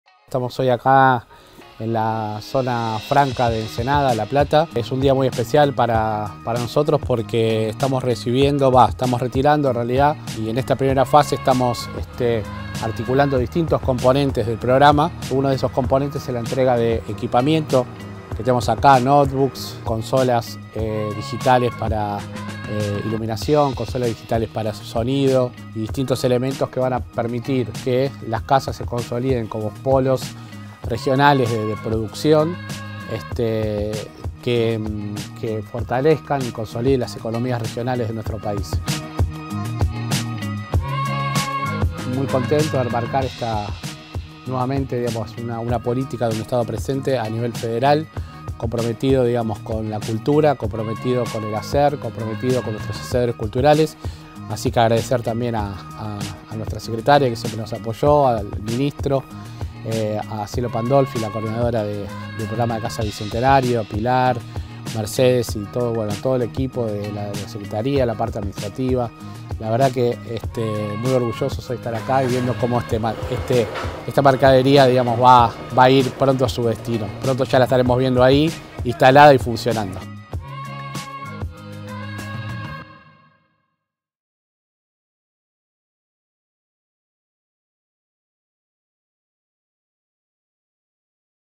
Valorización y Consolidación Regional de las Casas de la Historia y la Cultura del Bicentenario"
¡Seguimos creciendo!

Continuamos poniendo en marcha el programa "Valorización y Consolidación Regional de las Casas de la Historia y la Cultura del Bicentenario"

Recibimos 75 kits de equipamiento cultural destinados a 75 Casas en 21 provincias que incluyen: notebooks, consolas digitales para iluminación y para sonido, entre otros dispositivos.

Además, seguimos trabajando en el desarrollo de la plataforma en conjunto con ARSAT para las Casas de última tecnología en pos de habilitar y facilitar el acceso a capacitaciones profesionales en distintas disciplinas para la comunidad.
De esta forma, por medio de las industrias culturales y con un Estado presente, continuamos fortaleciendo el Turismo cultural y las economías regionales de nuestro país.